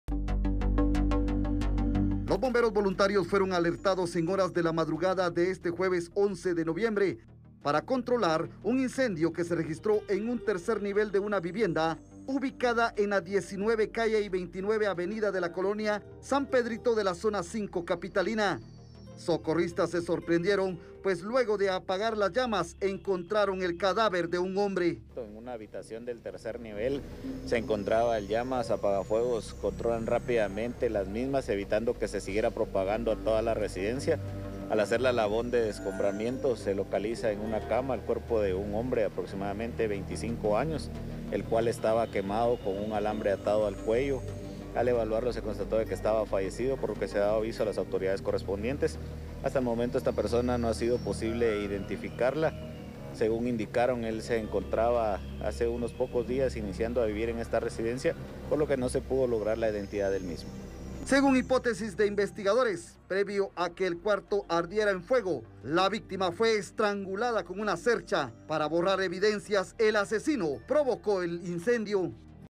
Hallan carbonizado el cuerpo de un hombre en incendio de vivienda en zona 5 capitalina
La víctima habría sido estrangulada antes del incendio de la vivienda

Una persona fue asesinada y luego prendida en llamas dentro de su vivienda. Los criminales, lo habrían hecho con la finalidad de borrar todo tipo de evidencia.